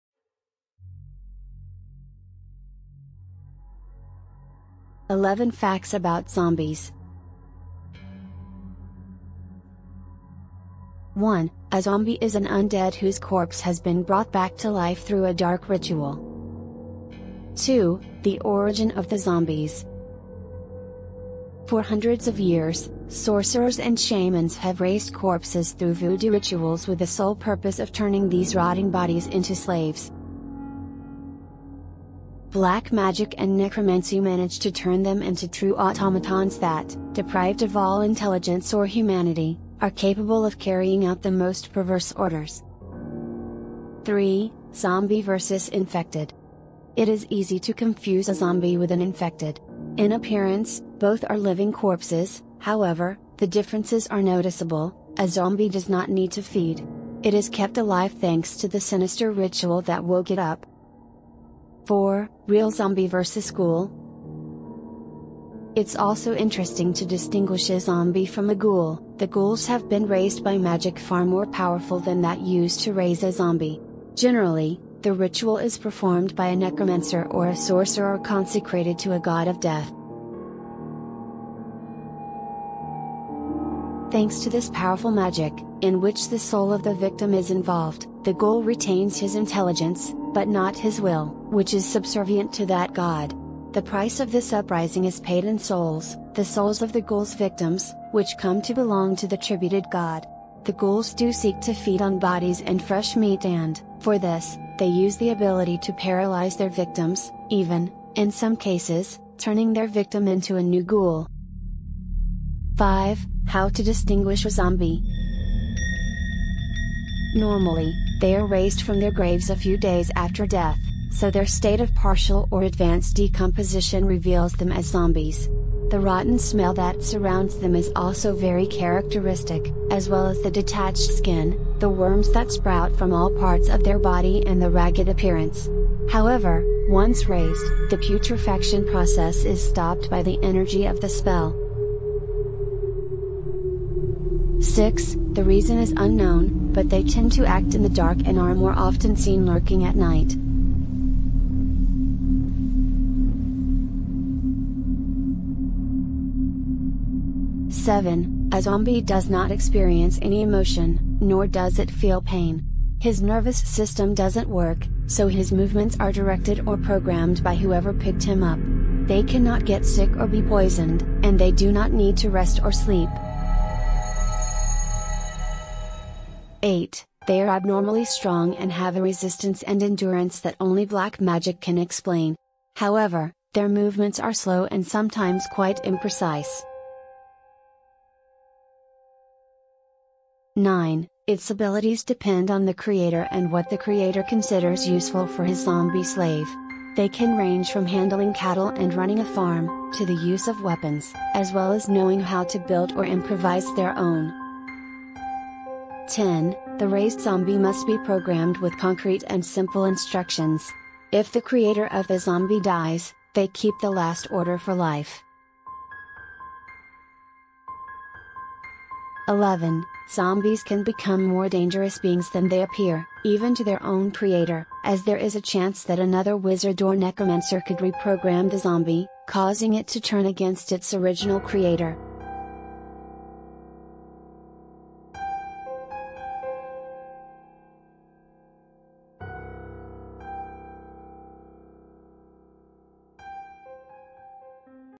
11 FACTS ABOUT ZOMBIES
Zombies can become more dangerous beings than they appear, even to their own creator, as there is a chance that another wizard or necromancer could reprogram the zombie, causing it to turn against its original creator.